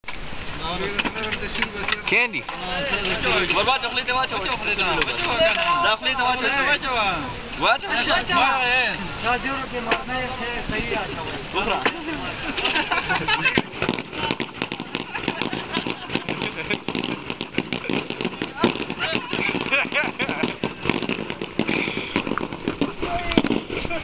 Afghan tries pop rocks
I'm in afghanistan, my wife sent me pop rocks, so I did what any well adjusted person would do